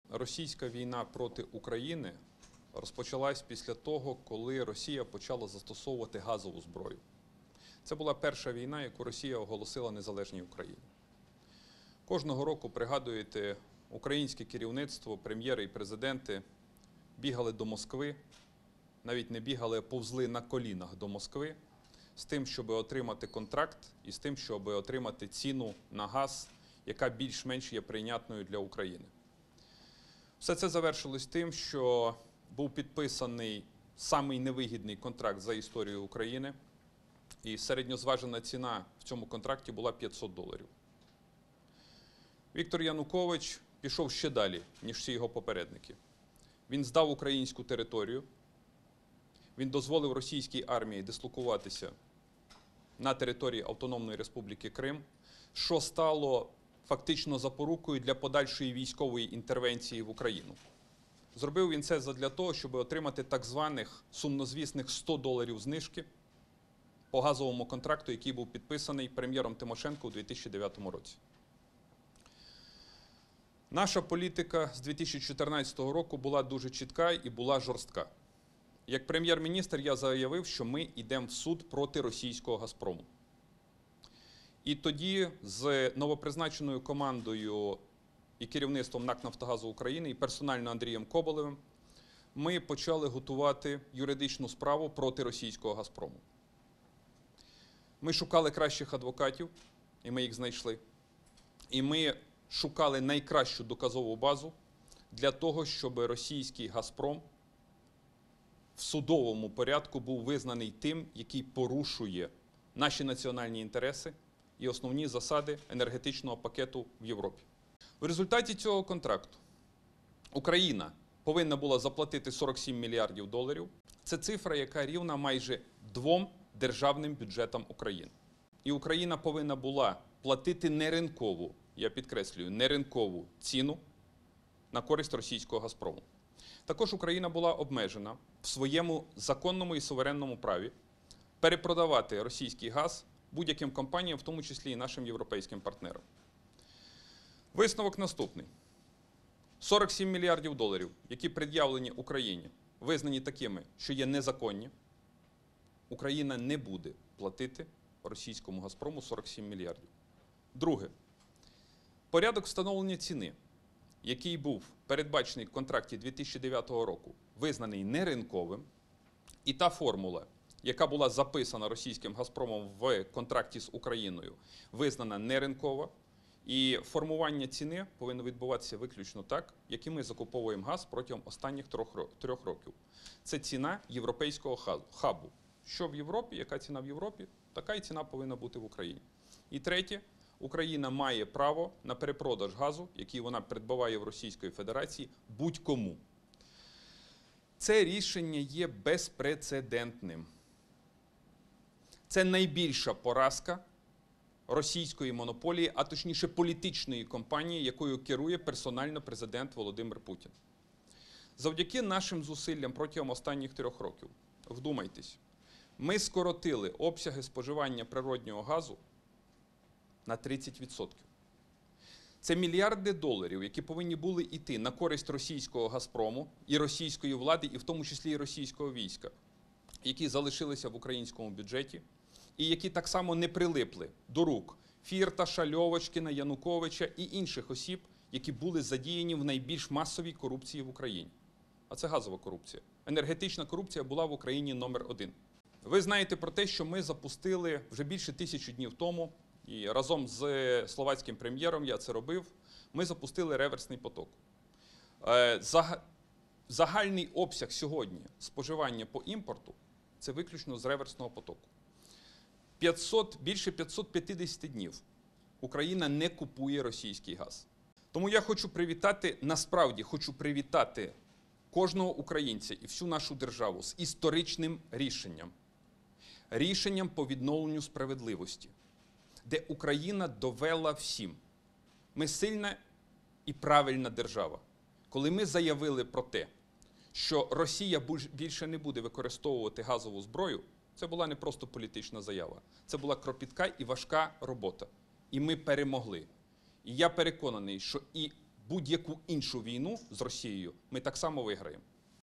Рішення Стокгольмського арбітражу – це найбільша поразка Путіна, - А.Яценюк
Рішення Стокгольмського арбітражного суду у позові «Нафтогазу» проти російського «Газпрому» є безпрецедентним: «Це найбільша поразка російської монополії, а точніше – політичної компанії, якою керує персонально Путін», - наголосив лідер політичної партії «Народний фронт» Арсеній Яценюк в інтерв’ю журналістам у п’ятницю, 2 червня.

Він підкреслив, що російська війна проти України розпочалася «після того, як Росія почала застосовувати газову зброю»: «Це була перша війна, яку Росія оголосила незалежній Україні. Кожного року українське керівництво, прем’єри і президенти  повзли на колінах до Москви, щоби отримати ціну на газ, яка є більш-менш прийнятною для України».

«Віктор Янукович пішов ще далі, ніж всі його попередники, - він здав українську територію, дозволив російській армії дислокуватися на території Автономної Республіки Крим, що стало фактично запорукою для подальшої військової інтервенції в Україну.

Арсеній Яценюк підкреслив, що з 2014 року політика української влади «була дуже чітка і жорстка: «Як прем’єр-міністр я заявив, що ми йдемо в суд проти російського «Газпрому». І тоді з новопризначеною командою і керівництвом НАК «Нафтогаз України» і персонально його головою Андрієм Коболєвим ми почали готувати юридичну справу проти російського «Газпрому».

«Ми шукали найкращих адвокатів, і ми їх знайшли. Ми збирали найкращу доказову базу для того, щоб російський «Газпром» в судовому порядку був визнаний тим, який порушує наші національні інтереси і основні засади Третього енергетичного пакету в Європі», - підкреслив лідер «Народного фронту».

 «Після того, як ми потягнули російський «Газпром» до суду, були різні думки, хтось був скептичний… А я був переконаний, що ми з командою «Нафтогазу» докажемо свою позицію і доведемо всьому світу, що російський «Газпром» - це не просто енергетична компанія, це політична компанія, яка використовує свій вплив, аби в тому числі позбавити Україну частини свого суверенітету і незалежності через неймовірні ціни і через жахливі, грабіжницькі умови цього контракту», - наголосив він.

У результаті цього контракту, підкреслив лідер «Народного фронту», Україна повинна була заплатити російському «Газпрому» $47 млрд: «Це цифра, яка рівна майже двом державним бюджетам України».

«Україна повинна була платити неринкову ціну на користь російського «Газпрому», а також обмежена в своєму законному і суверенному праві перепродавати російський газ будь-яким компаніям, в тому числі європейським партнерам», - нагадав він.

Завдяки рішенню Стокгольмського арбітражу Україна не буде платити російському «Газпрому» $47 млрд, підкреслив Арсеній Яценюк: «Порядок встановлення ціни, який був передбачений в контракті 2009 року, визнаний неринковим. Формула, яка була записана російським «Газпромом» у контракті з Україною, визнана неринковою. Формування ціни повинне відбуватися так, як ми і закуповуємо газ протягом останніх трьох років. Це ціна європейського хабу. Яка ціна в Європі – така ціна повинна бути і в Україні».

Крім того, за рішенням суду, Україна має право на перепродаж газу, який вона придбаває в Російської Федерації, будь-яким іншим компаніям.

Лідер «Народного фронту» також підкреслив, що завдяки зусиллям останніх трьох років, обсяги споживання природнього газу в Україні скорочені на 30%: «Це мільярди доларів, які повинні були йти на користь російського «Газпрому», російської влади і в тому числі – російського війська. Ці кошти залишилися в українському бюджеті і  не прилипли до рук Фірташа, Льовочкіна, Януковича і інших осіб, які були задіяні в найбільшій корупції в Україні, адже енергетична корупція – це була корупція номер один».

Він також нагадав, що понад тисячу днів тому він на посту Глави Уряду разом з прем’єр-міністром Словаччини дав старт реверсному потоку газу: «Сьогодні загальний обсяг споживання газу по імпорту – це виключно з реверсного потоку. Понад 550 днів Україна не купує російський газ».

«Хочу привітати кожного українця і всю нашу державу з історичним рішенням - з відновленням справедливості. Україна довела всім – ми сильна держава. Коли ми заявили про те, що Росія більше не буде використовувати газову зброю – це була не просто заява. Це була кропітка і важка робота. І ми перемогли. Переконаний, що і будь-яку іншу війну з Росією ми так само виграємо», - сказав Арсеній Яценюк.

Приєднуйся!